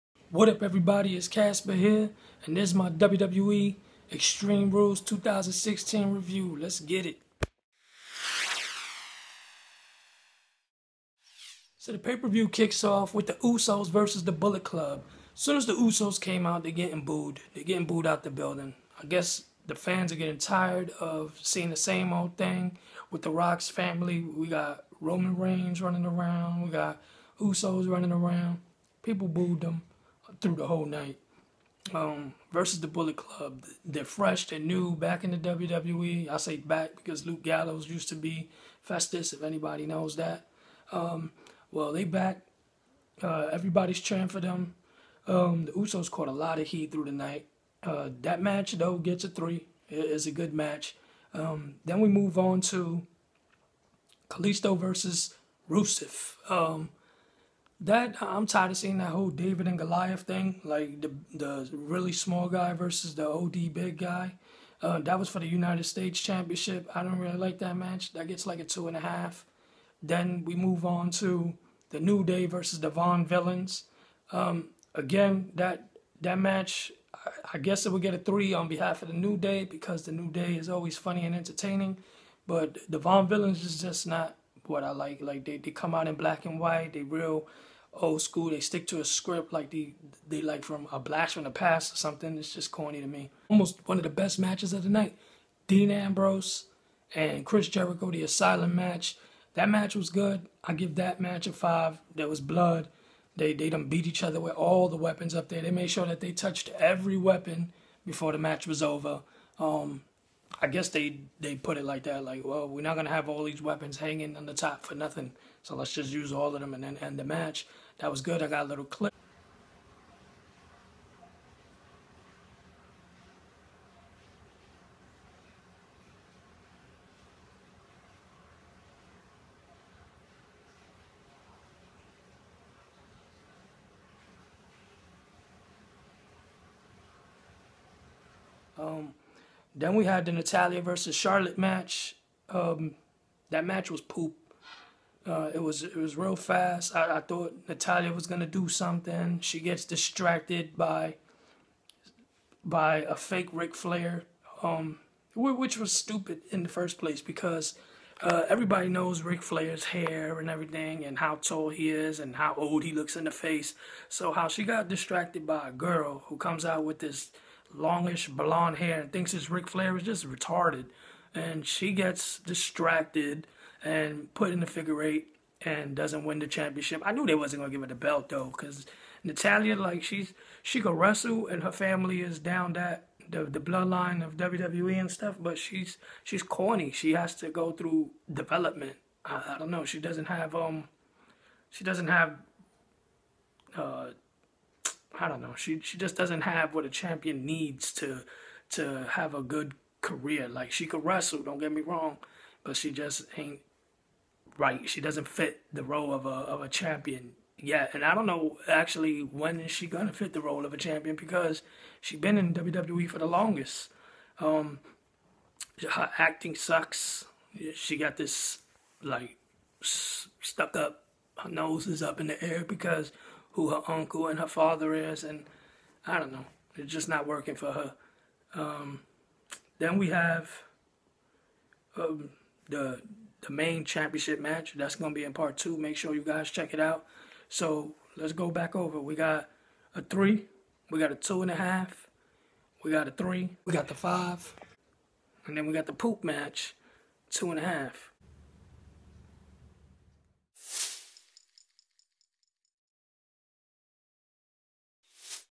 WWE EXTREME RULES 2016 REVIEW SETH ROLLINS RETURN Reaction
WWE EXTREME RULES
The RETURN OF SETH ROLLINS
AJ STYLES VS ROMAN REIGNS
And more !